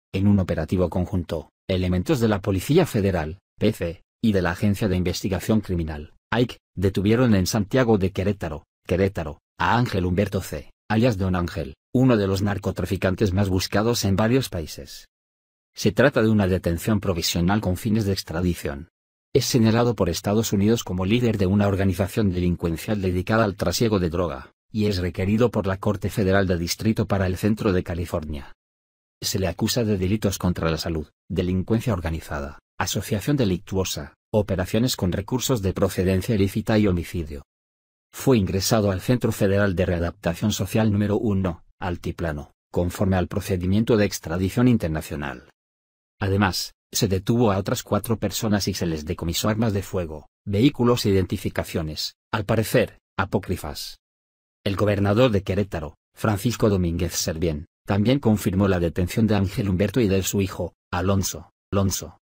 Detienen en Querétaro a narcotraficante buscado en varios países
En un operativo conjunto, elementos de la Policía Federal (PF) y de la Agencia de Investigación Criminal (AIC) detuvieron en Santiago de Querétaro, Querétaro, a Ángel Humberto "C", alias "Don Ángel", uno de los narcotraficantes más buscados en varios países.

Se trata de una detención provisional con fines de extradición. Es señalado por Estados Unidos como líder de una organización delincuencial dedicada al trasiego de droga, y es requerido por la Corte Federal de Distrito para el Centro de California.